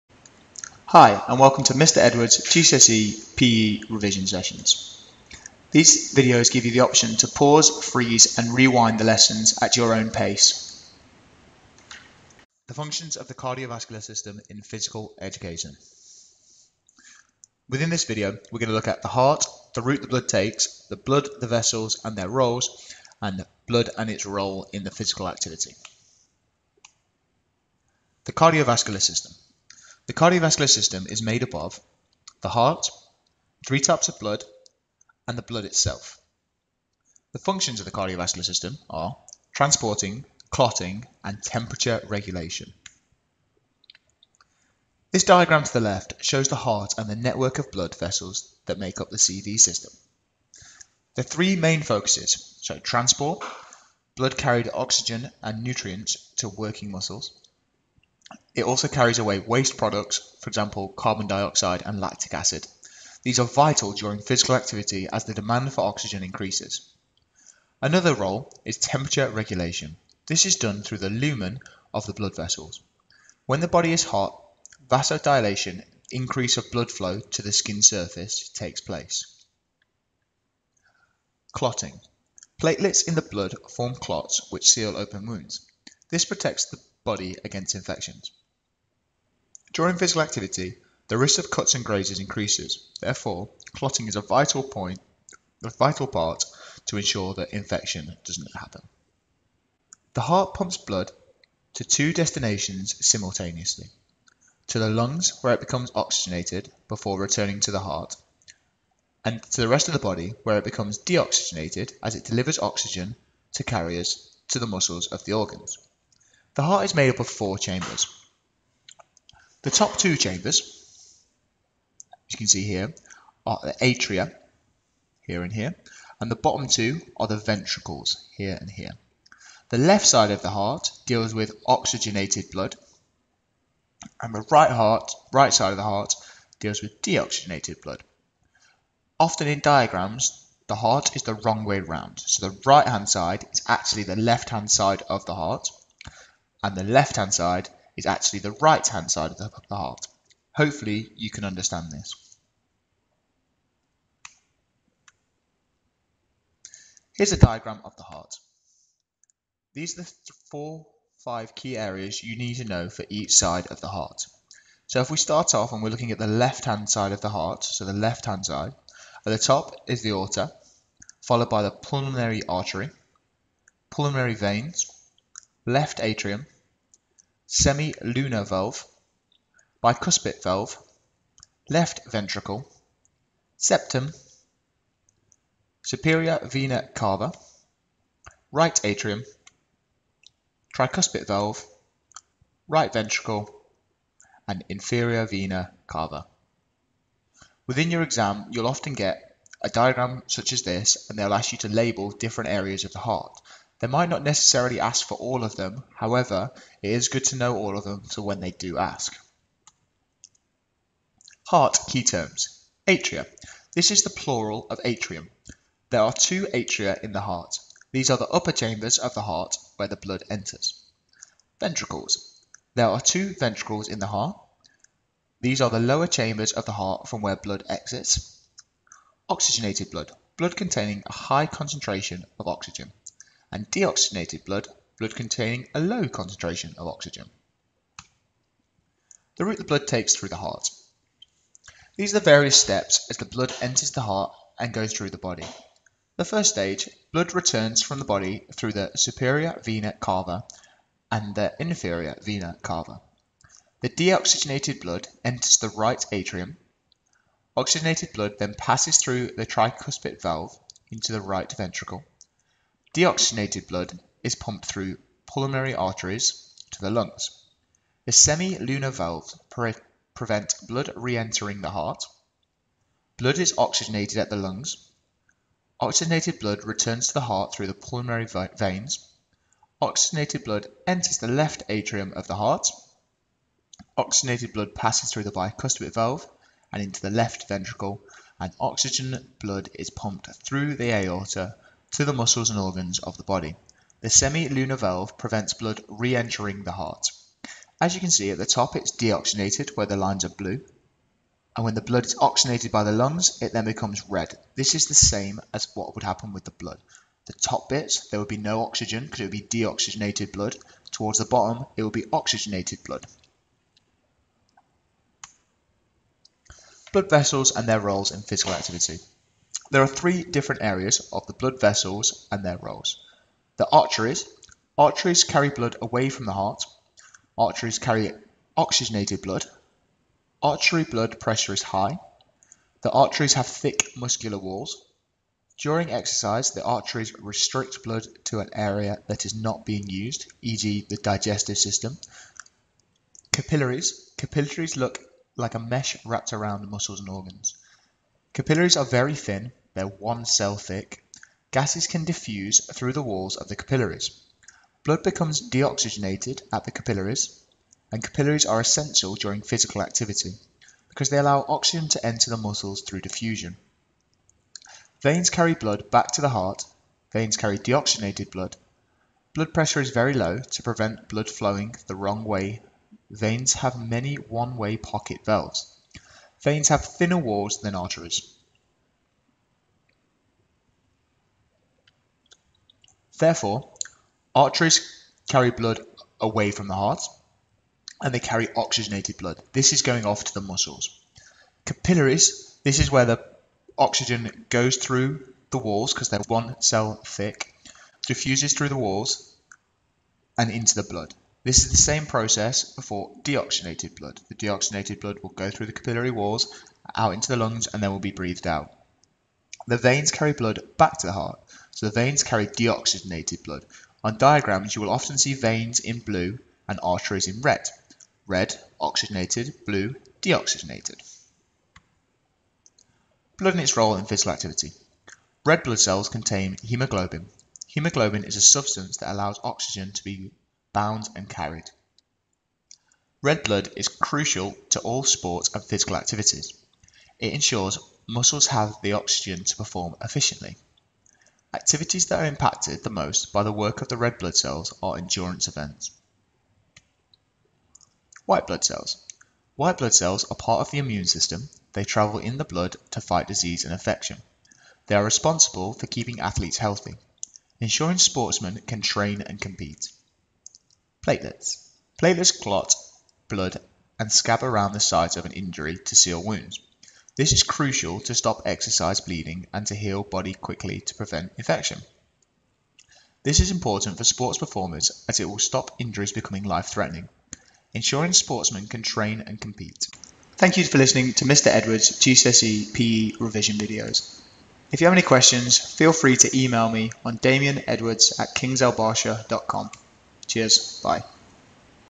GCSE PE – Lesson 15 - The Functions of the Cardiovascular System In Physical Education
GCSE PE flipped learning video -

Classification and characteristics of muscle fibre types, Athlete examples and summaries of each one

Table of Contents:

00:25 - The cardio vascular system
00:42 -
02:34 -
03:29 - Heart Key terms
03:57 - The route the blood takes through the heart
05:15 - Blood vessels and their role in physical activity
07:01 - Blood and its role in physical activity
07:26 - Blood and its role in physical activity
07:39 - Blood and its role in physical activity